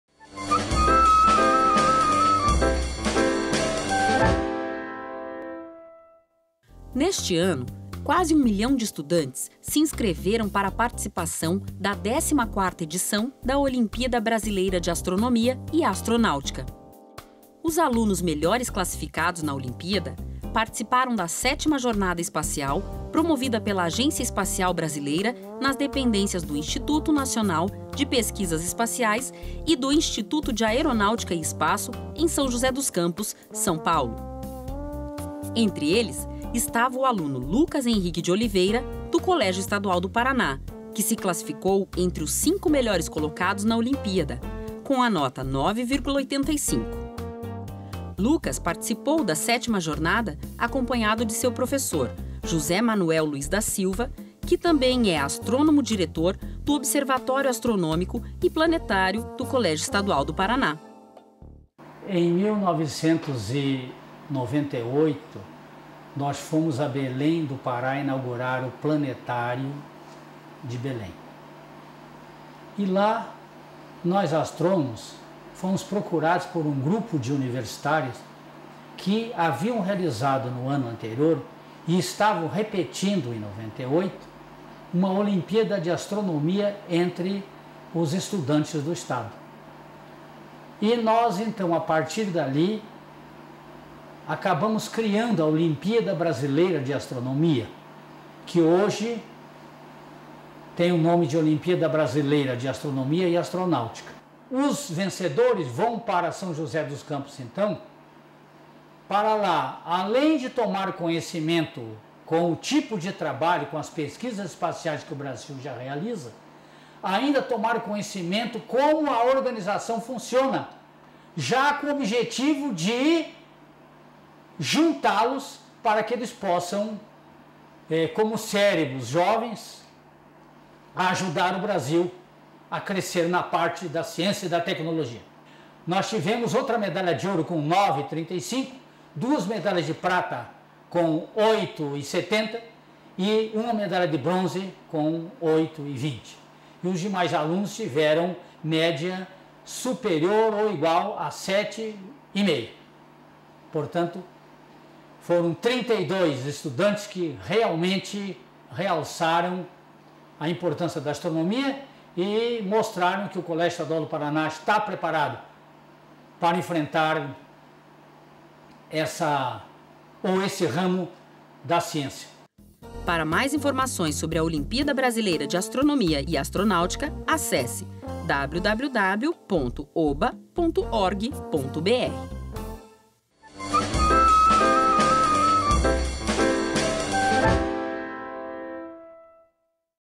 Extraclasse - Olímpiada Brasileira de Astronomia e Astronáutica
XIV Olimpíada Brasileira de Astronomia e Astronáutica. Sinopse: O Extraclasse 66 destaca a premiação do aluno do CEP, Lucas Henrique de Oliveira, na XIV Olimpíada Brasileira de Astronomia e Astronáutica. O informativo apresenta fotos da premiação do aluno, no próprio CEP.